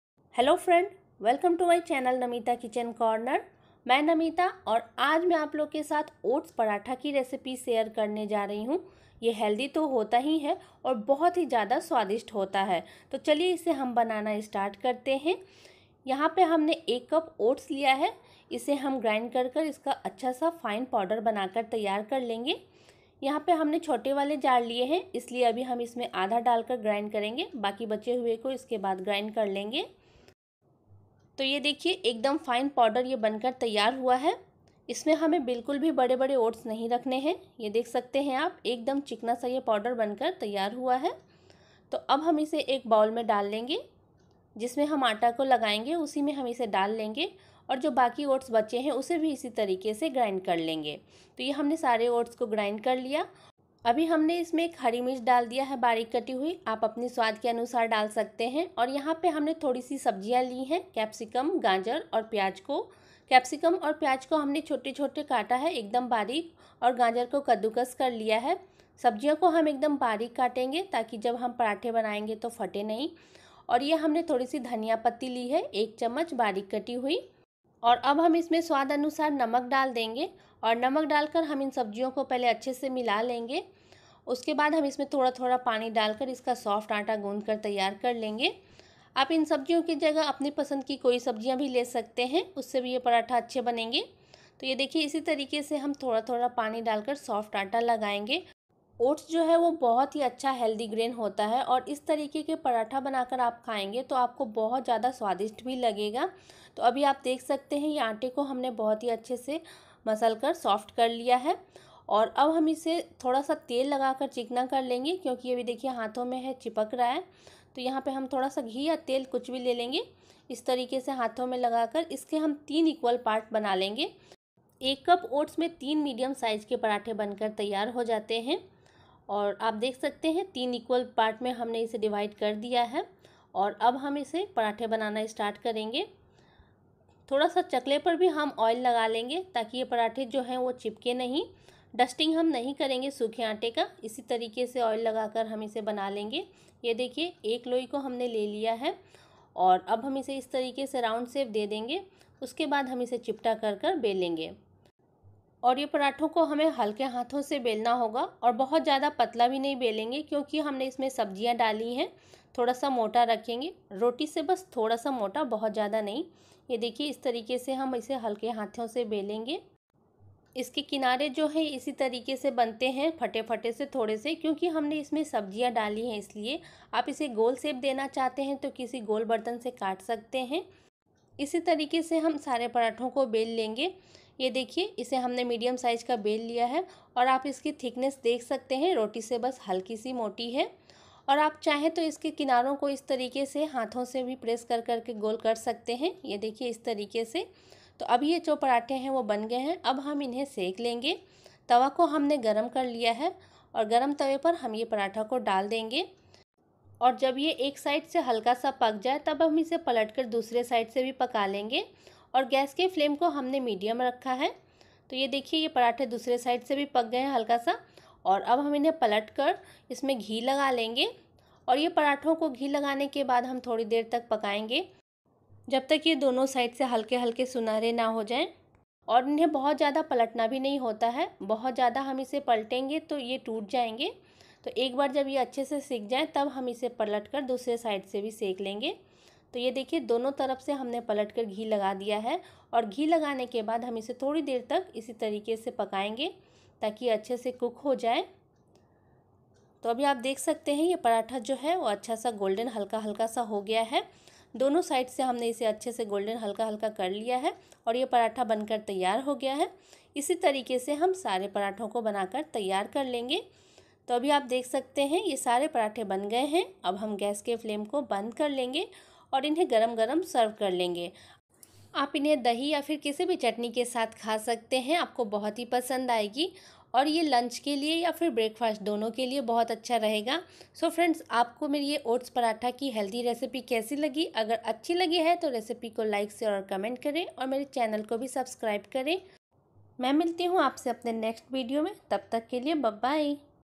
oats paratha recipe | If u have oats try this spicy paratha | easy breakfast | ओट्स
Friends es video me maine oats paratha ki recipe share ki hai hope aplog ko pasand aae hogi.

oats paratha
oats recipe
paratha recipe
healthy breakfast recipe
ओट्स रेसिपी
ओट्स पराठा

ingradients:-

oats / jaee
carrot / gajar
capsicum / simla mirch
onion / pyaj
ghee
salt / namak
water / pani
green chili / hari mirch
coriander leaves / dhaniya patti